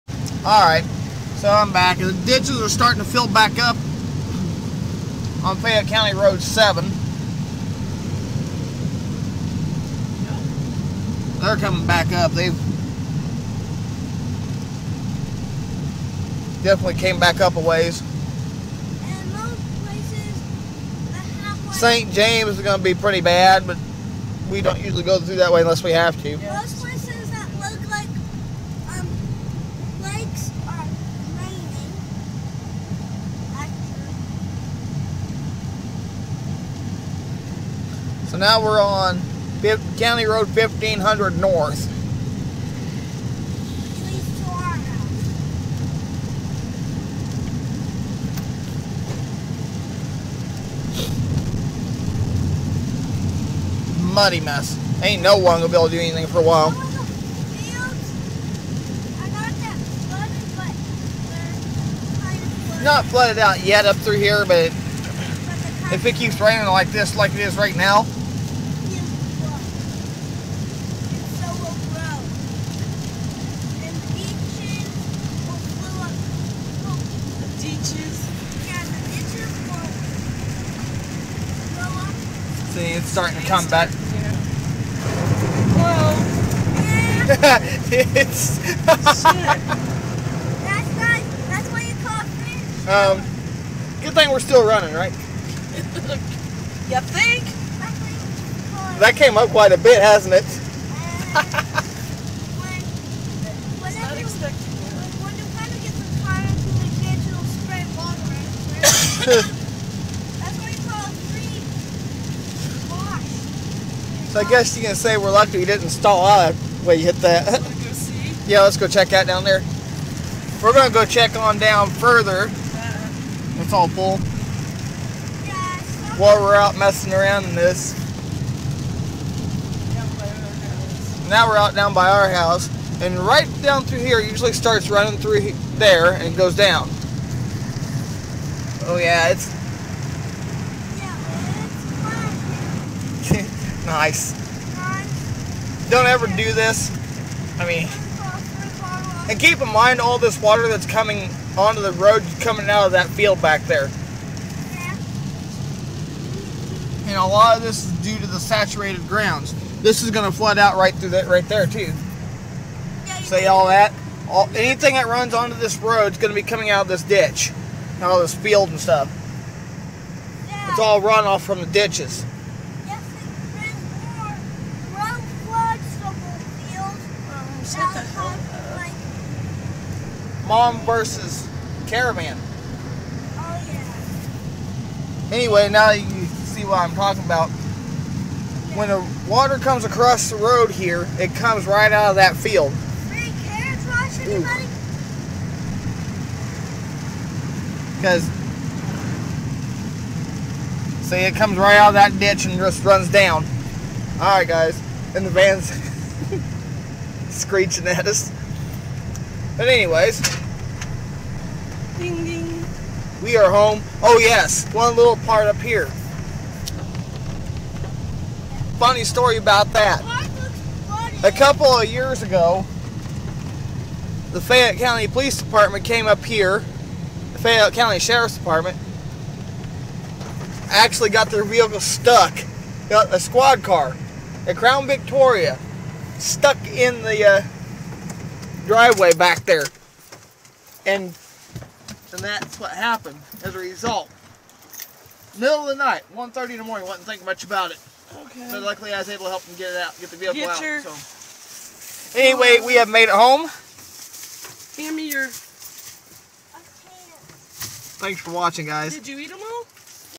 WASNT EXPECTING THAT GOOD THING WE ARE STILL RUNNING YA THINK?
THIS WAS ON COUNTY ROAD 15OO NORTH IN RURAL ST ELMO SHOWIN CERTAIN SECTIONS OF THE ROAD THAT FLOOD WELL EPLAINING ANYWAYS  PLOWED RIGHT THROUGH ONE SPOT KKIND OF CAUGHT MY MOM OFF GARD  LUCKY WE DIDNT STALL OUT RIGHT THERE